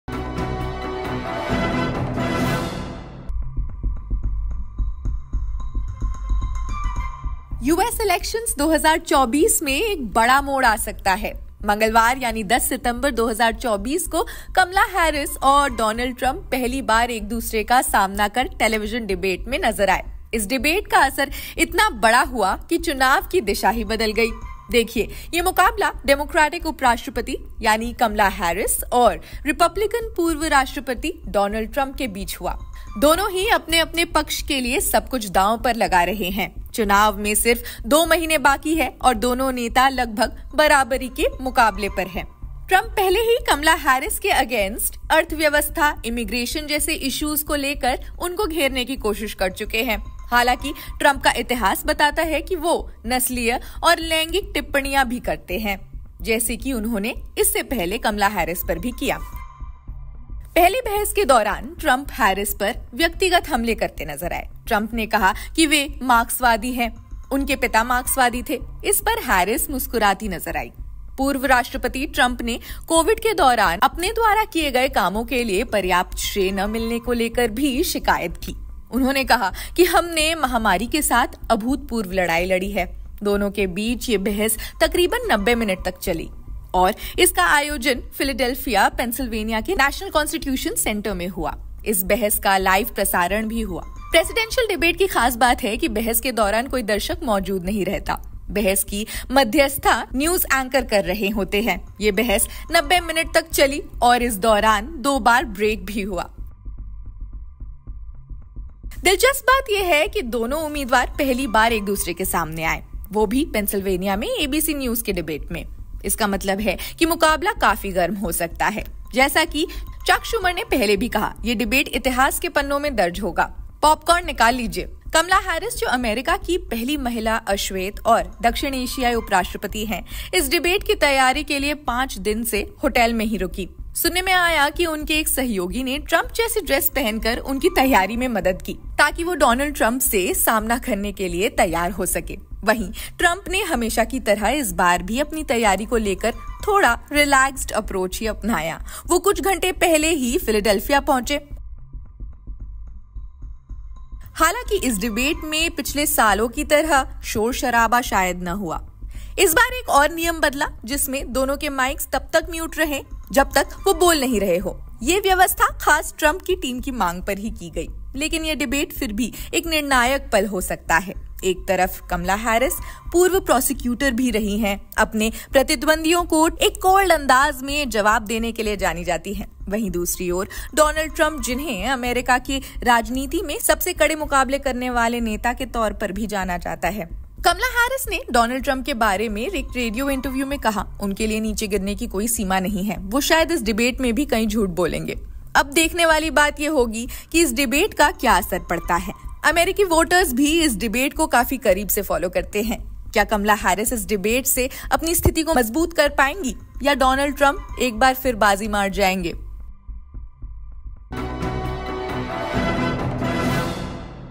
Kamala Harris VS Trump: Who is ahead and who is behind in President Debate, Kamala cornered Donald
US Elections Presidential Debate: During the debate, Trump was also seen making personal attacks on Kamala Harris. Trump said he is a Marxist. His father was a Marxist. Harris was seen smiling at this. Former President Trump also complained about not getting enough credit for the work he did during Covid.